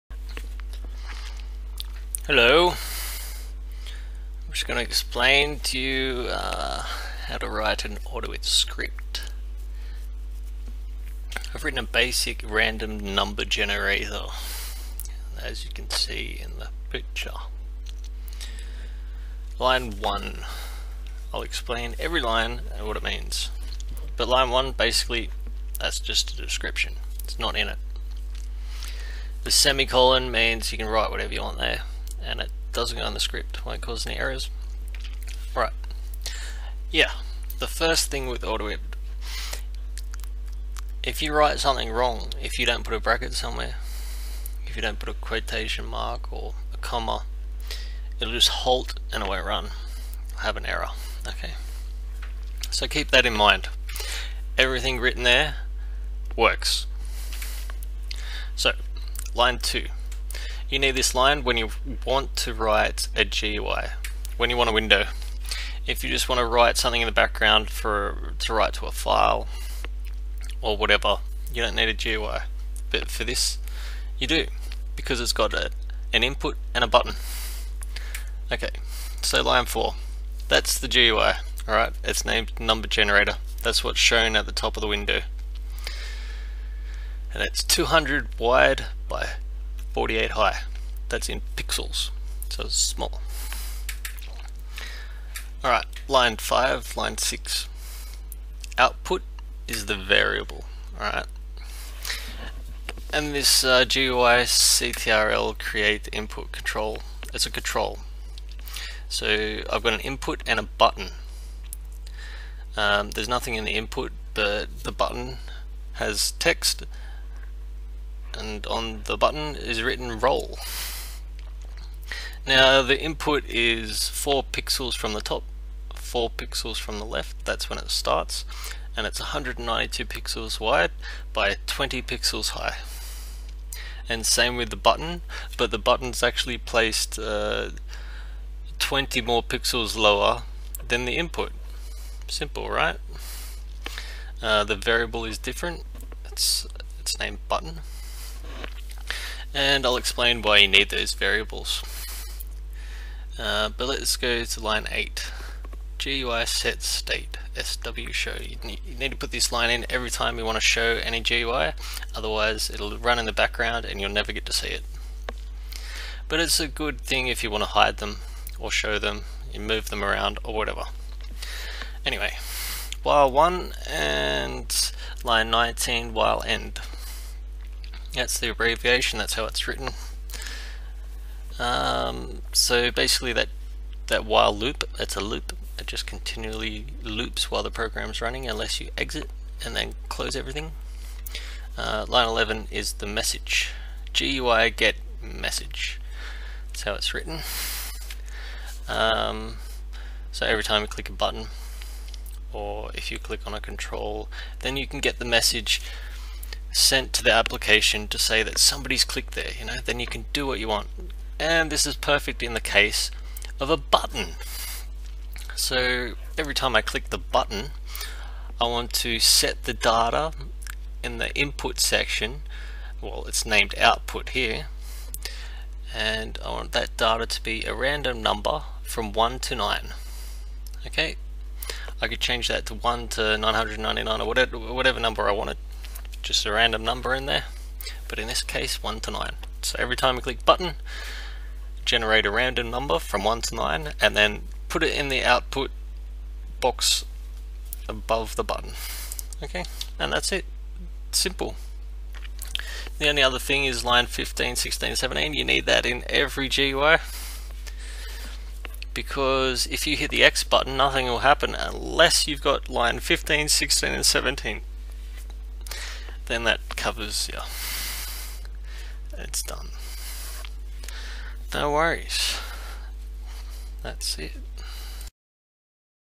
This is how to write a random number script/gui with autoit scripting technology.
This is a guide for beginners. AutoIt has many features for beginners and it is quite easy to create/compile a script. Make your own customised windows.